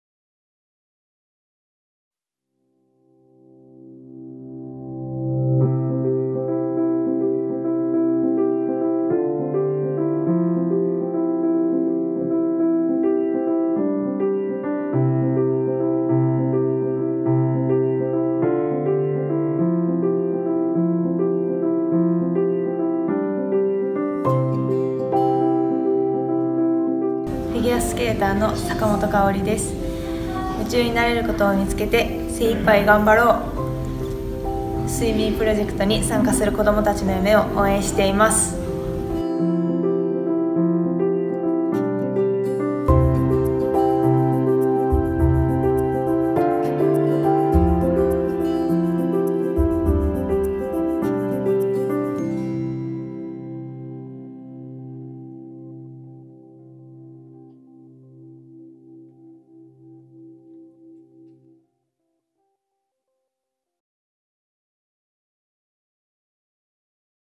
スイミープロジェクト I’ll be the eye フィギュアスケート選手坂本花織さん
神戸出身のフィギュアスケート選手坂本花織さんからスイミープロジェクトに参加する子ども達へ応援のメッセージをいただきました。

『夢中になれることをみつけて精一杯頑張ろう』

【スイミープロジェクト】
巨大こいのぼりの制作・掲揚を通して、子ども達の夢と成長を応援する活動。

【I’ll be the eye イベント】
こいのぼりの制作・掲揚を通して、子ども達の成長と夢を応援する「スイミープロジェクト」の活動の一環として、絵本「スイミー」で黒い魚スイミーが仲間達の先頭にっ立って、 励まそうとした言葉「ぼくがめになろう」になぞらえて、 各分野で活躍する方々に参加する子どもたちへ夢を育む事へのメッセージをスイミーこいのぼり（全長10m）の大きな目に頂きました。
子ども達がメッセージを受けて、自分の夢の描き方を考えるきっかけになればと思います。